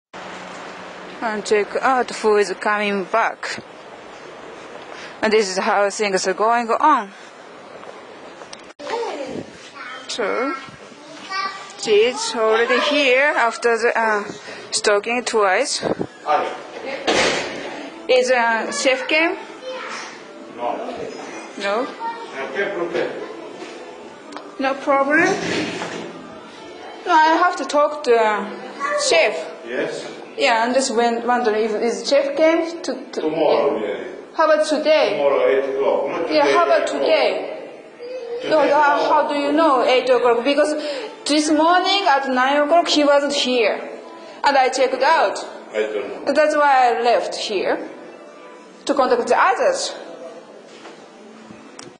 Resource: A perp in my refugee residence (02/28/08)
Just like the group I saw in the Kanton library and on the street. I saw him several times on Feb 28th '08.